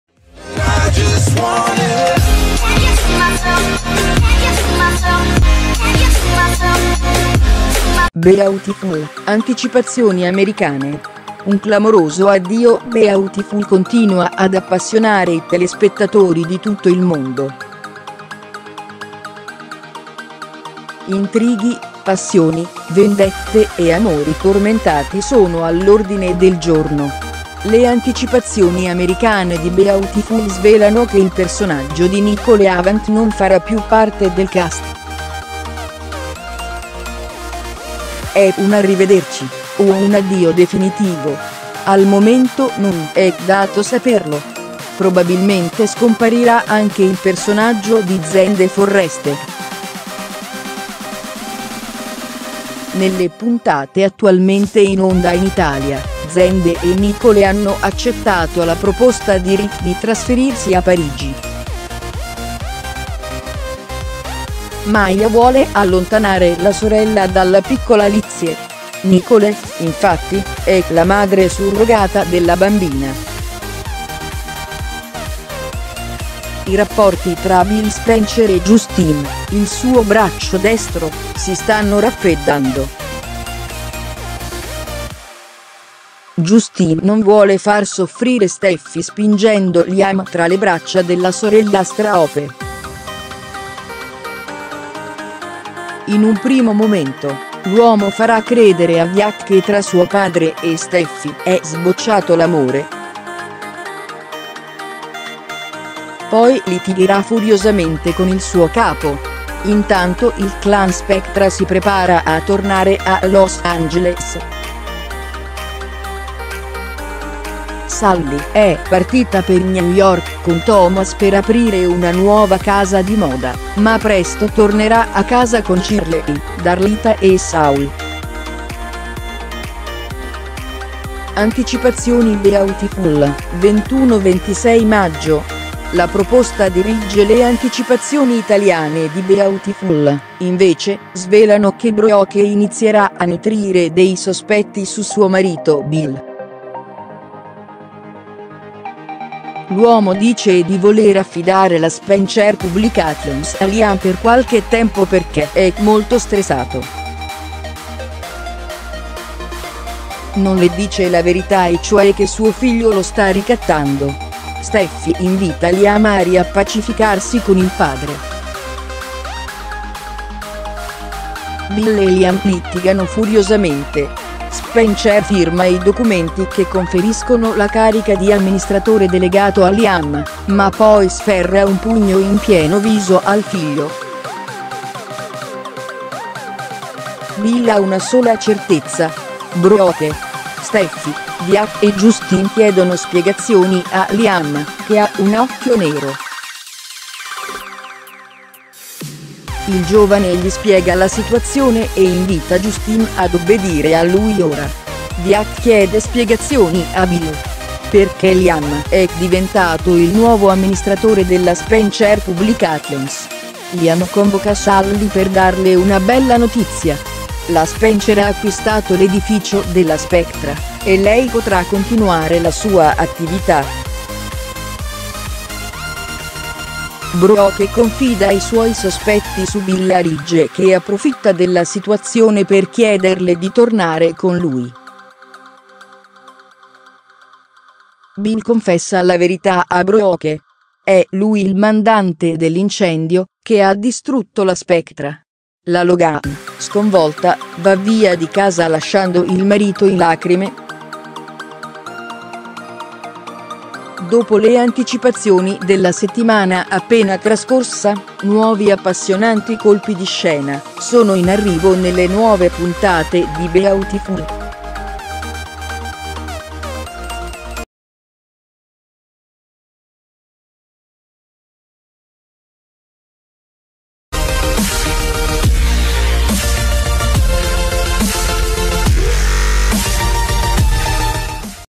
Beautiful, anticipazioni 21-26 maggio e puntate americane
Beautiful, anticipazioni americane: un clamoroso addio  Beautiful continua ad appassionare i telespettatori di tutto il mondo. Intrighi, passioni, vendette e amori tormentati sono all’ordine del giorno. Le anticipazioni americane di Beautiful svelano che il personaggio di Nicole Avant non farà più parte del cast. E’ un arrivederci o un addio definitivo? Al momento non è dato saperlo. Probabilmente scomparirà anche il personaggio di Zende Forrester. Nelle puntate attualmente in onda in Italia, Zende e Nicole hanno accettato la proposta di Rick di trasferirsi a Parigi. Maya vuole allontanare la sorella dalla piccola Lizzie. Nicole, infatti, è la madre surrogata della bambina. I rapporti tra Bill Spencer e Justin, il suo braccio destro, si stanno raffreddando. Justin non vuole far soffrire Steffy spingendo Liam tra le braccia della sorellastra Hope. In un primo momento, l’uomo farà credere a Wyatt che tra suo padre e Steffy è sbocciato l’amore. Poi litigherà furiosamente con il suo capo. Intanto il clan Spectra si prepara a tornare a Los Angeles. Sally è partita per New York con Thomas per aprire una nuova casa di moda ma presto tornerà a casa con Shirley, Darlita e Saul.  Anticipazioni Beautiful, 21-26 maggio: la proposta di Ridge  Le anticipazioni italiane di Beautiful, invece, svelano che Brooke inizierà a nutrire dei sospetti su suo marito Bill. L’uomo dice di voler affidare la Spencer Publications a Liam per qualche tempo perchè è molto stressato. Non le dice la verità e cioè che suo figlio lo sta ricattando. Steffy invita Liam a riappacificarsi con il padre. Bill e Liam litigano furiosamente. Spencer firma i documenti che conferiscono la carica di amministratore delegato a Liam ma poi sferra un pugno in pieno viso al figlio. Bill ha una sola certezza: Brooke! Steffy, Wyatt e Justin chiedono spiegazioni a Liam che ha un occhio nero. Il giovane gli spiega la situazione e invita Justin ad obbedire a lui ora. Wyatt chiede spiegazioni a Bill: perchè Liam è diventato il nuovo amministratore della Spencer Publications? Liam convoca Sally per darle una bella notizia: la Spencer ha acquistato l’edificio della Spectra e lei potrà continuare la sua attività. Brooke confida i suoi sospetti su Bill a Ridge che approfitta della situazione per chiederle di tornare con lui. Bill confessa la verità a Brooke: è lui il mandante dell’incendio che ha distrutto la Spectra! La Logan, sconvolta, va via di casa lasciando il marito in lacrime. Dopo le anticipazioni della settimana appena trascorsa, nuovi appassionanti colpi di scena sono in arrivo nelle nuove puntate di Beautiful.